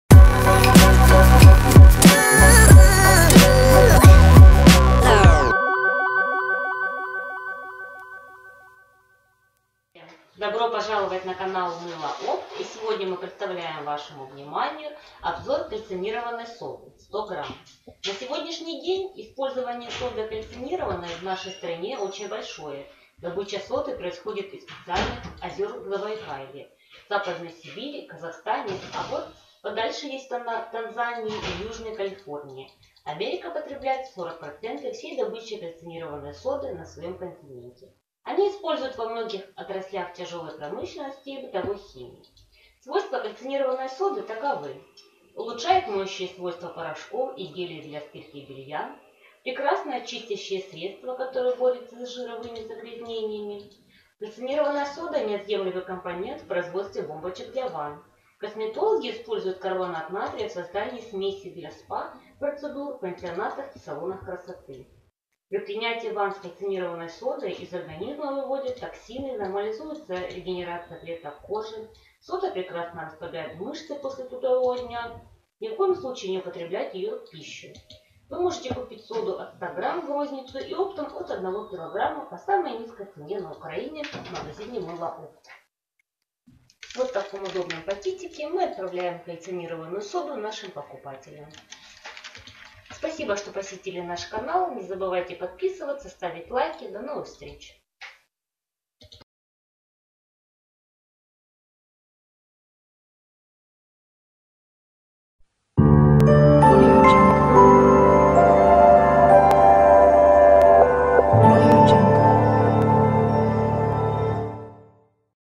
Кальцинированная сода// Сода в косметике// Сода для похудения
Видео обзор Кальцинированная сода от магазина Мыло-опт